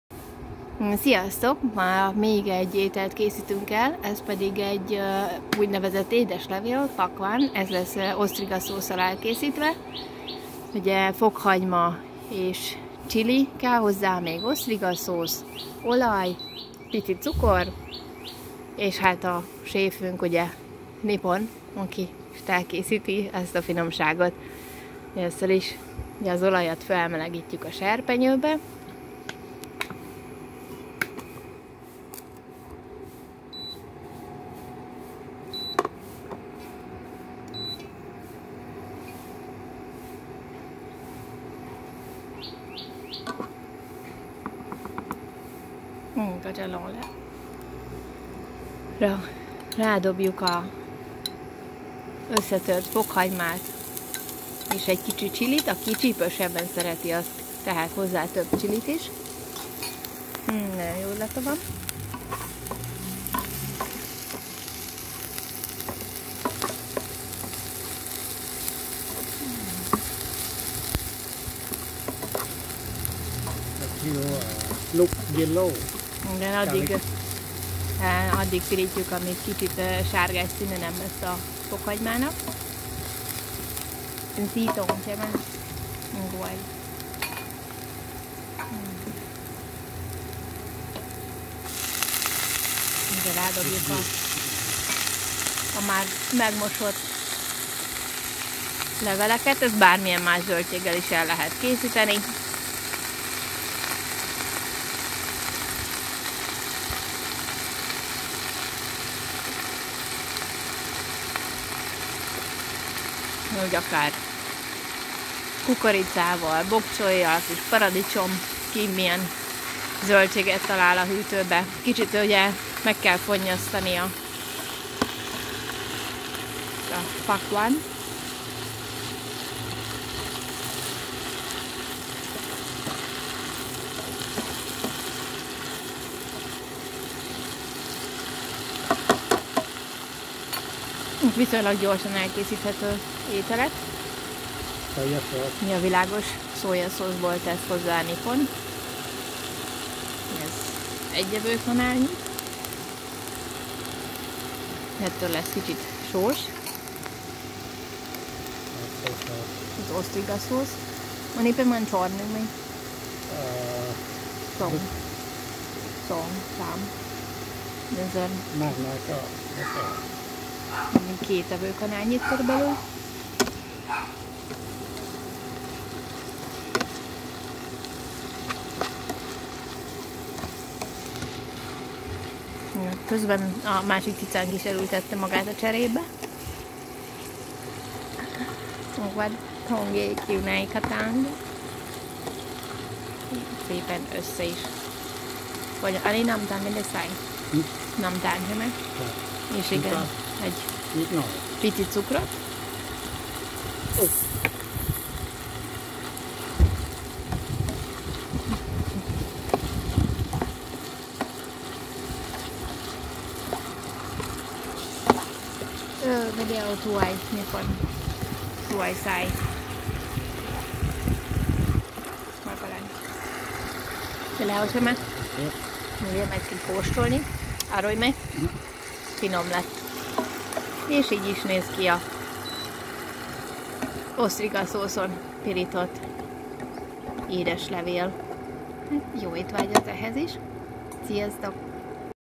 Pak Whan elkészítése (ผักหวาน)
A Phak Wan (   EUPHORBIACEAE:Sauropus abicans) olyan  thai zöldség, amely sokféle étel főzéséhez használható.  Magas tápértékű gyógynövény , fehérjeforrás, C-vitamin és béta-karotin tartalma  segíti a látást, táplálja a szemet és antioxidáns tulajdonságokkal is rendelkezik.  Magas a kalciumban és a foszforban.  Segít fenntartani az erős csontokat és fogakat.